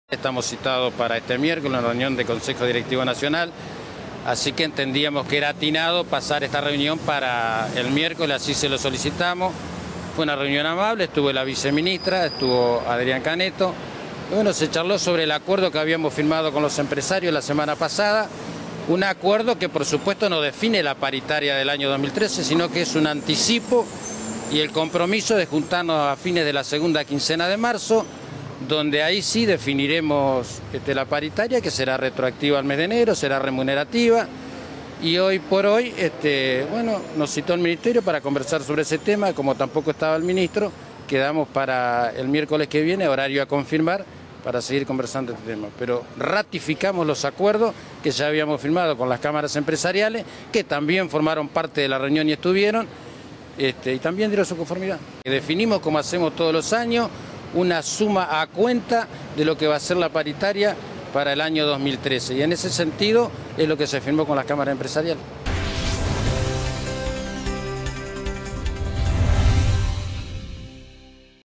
Gremios, cámaras y trabajo se reunirán nuevamente el miércoles
El Ministerio de Trabajo y la Asociación Bancaria (AB) informaron esta tarde que los representantes paritarios del gremio y los directivos de las cámaras se reunirán nuevamente el miércoles próximo a las 11.
Las fuentes laborales y el secretario de Prensa del gremio, Eduardo Berrozpe, indicaron esta tarde que sindicalistas y empresarios fueron convocados para las 11 del día 23 en la sede ministerial de la Avenida Leandro N. Alem 650.
Berrozpe sostuvo que los representantes bancarios ratificaron hoy la solicitud de postergación del encuentro convocado por la cartera laboral para hoy y también "los acuerdos alcanzados sobre un anticipo de tres pagos de 1.444 pesos y una suma adicional no remunerativa de 770 pesos para el primer trimestre" del año.
La delegación paritaria del gremio será encabezada el miércoles por el secretario general de la organización, Sergio Palazzo, quien será acompañado por los integrantes del consejo directivo.
"Lo acordado la semana anterior no representó un acuerdo convencional paritario sino un anticipo a cuenta de futuros aumentos. Los números del convenio definitivo serán discutidos a partir de la segunda quincena de marzo", aseguró hoy Berrozpe.
El encuentro de hoy en Trabajo fue encabezado por la viceministra Noemí Rial, quien convocó a las partes nuevamente para el 23.
La Bancaria (AB) había solicitado al ministro Carlos Tomada "una postergación" del encuentro de hoy en una carta firmada por el secretario de Acción Gremial, Gustavo Díaz, quien indicó que "el Secretariado General Nacional del sindicato está integrado por un importante número de dirigentes del interior" del país.
Además, "el gremio convocó a reunión de consejo directivo para el  23, por lo que un gran número de dirigentes del interior llegarán para asistir a ese encuentro", señaló la Bancaria.